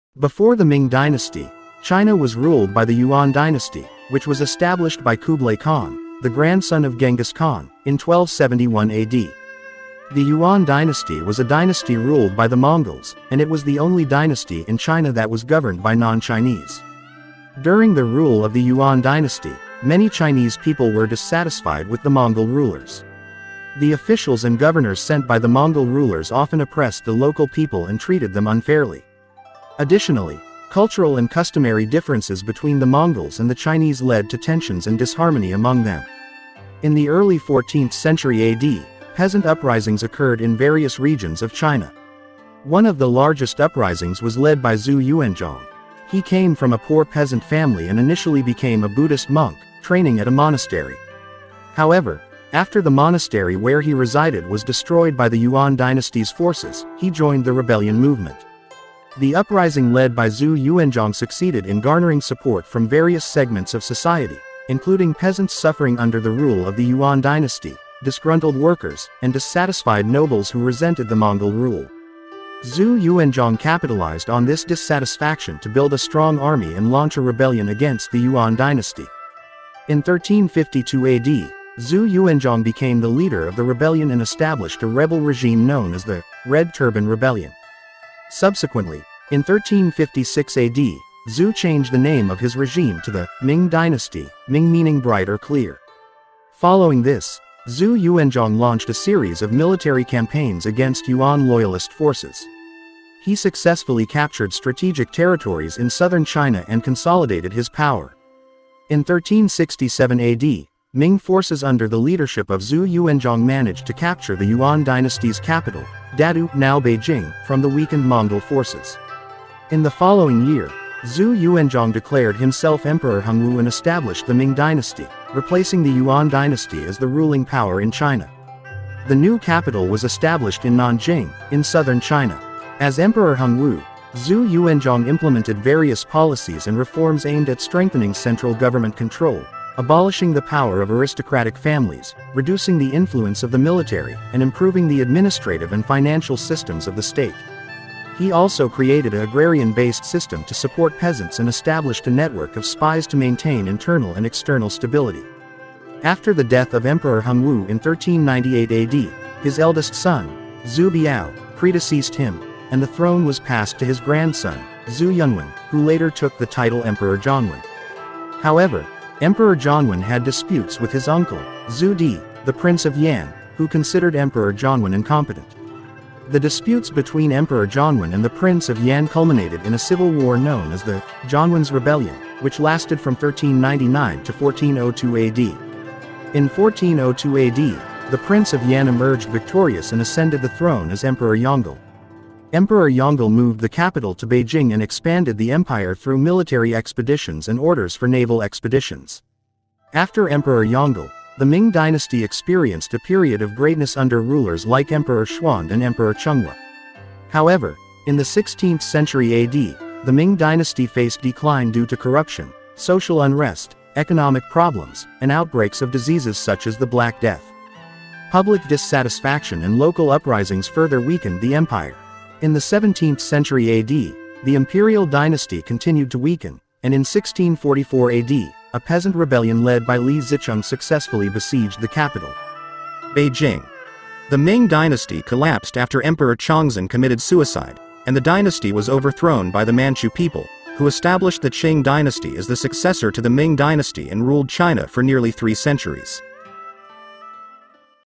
"Ming Dynasty: From Rebellion to Glory and the Tragic Fall of China's Last Imperial Power"
The Ming dynasty, officially the Great Ming, was an imperial dynasty of China, ruling from 1368 to 1644 following the collapse of the Mongol-led Yuan dynasty. The Ming dynasty was the last imperial dynasty of China ruled by the Han people, the majority ethnic group in China. Although the primary capital of Beijing fell in 1644 to a rebellion led by Li Zicheng (who established the short-lived Shun dynasty), numerous rump regimes ruled by remnants of the Ming imperial family—collectively called the Southern Ming—survived until 1662.